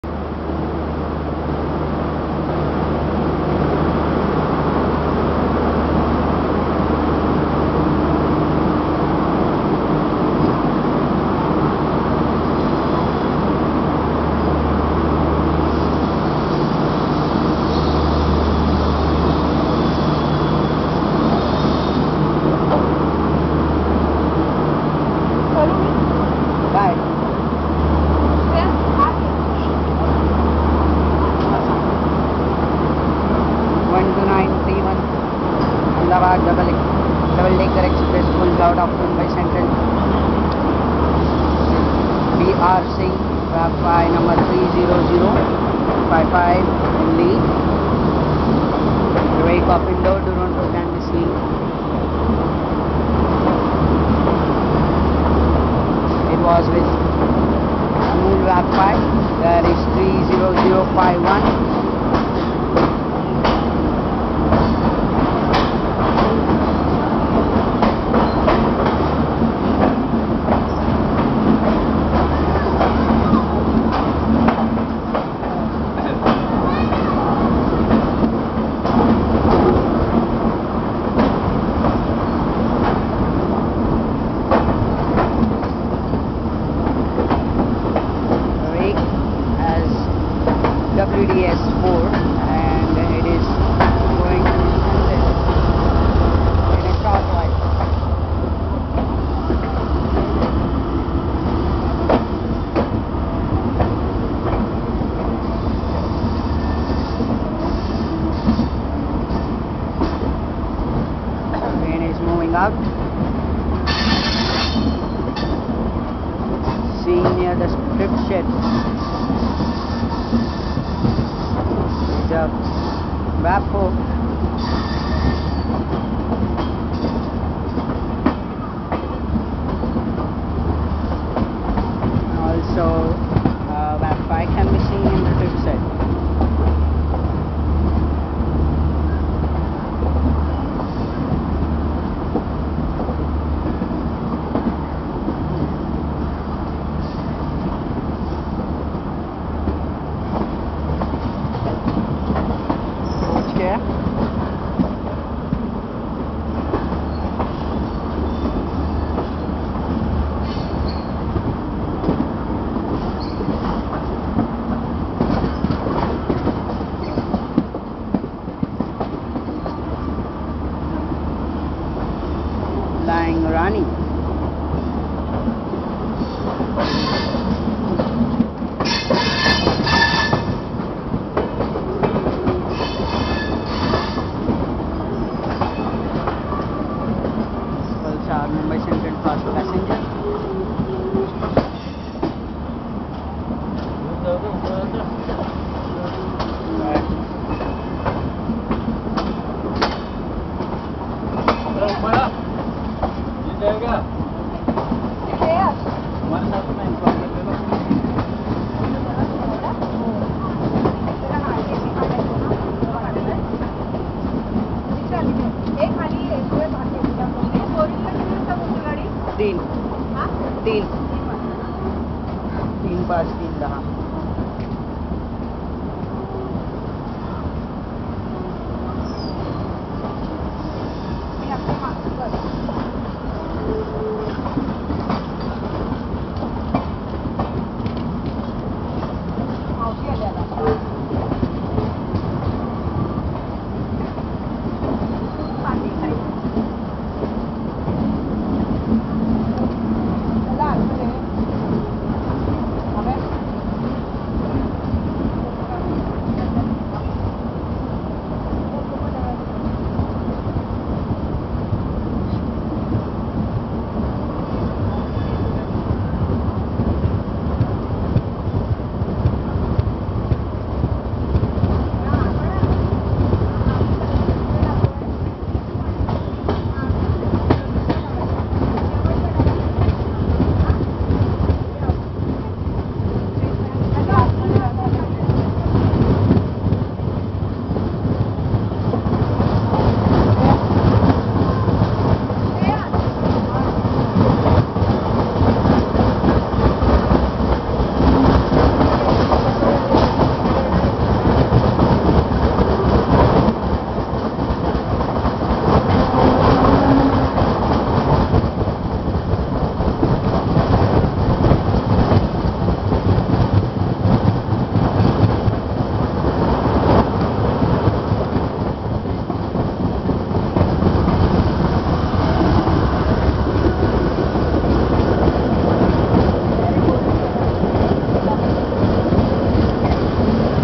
IRFCA - DEPARTING MUMBAI CENTRAL IN 12931 AC DD!
Time: 14:20
Date: 25 May 2013
Location: Mumbai Central to Mahalaxmi.
I am onboard 12931 BCT-ADI ACC DD and on the way to BRC. The report of the journey can seen here -